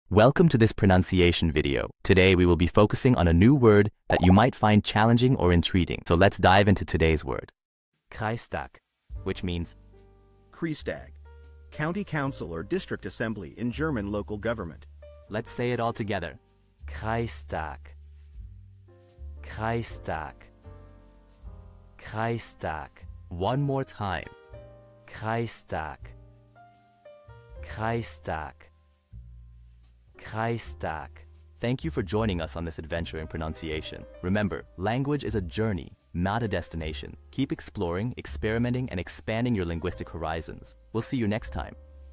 How to pronounce Kreistag
Master the Pronunciation of 'Kreistag which Means district council' - which means : District: County Council Or District Assembly in German Local Government. 📝 with @PronunciationGerman 🎓🗣️ - [Your Guide to German]

Learn to pronounce 'Kreistag' perfectly in German with our easy and engaging tutorial by @PronunciationGerman! 📖📽️ In this comprehensive video, we provide you with native speaker examples 🗣️👂 and phonetic breakdowns 🎵 to ensure you get the pronunciation just right.

How to say District council in German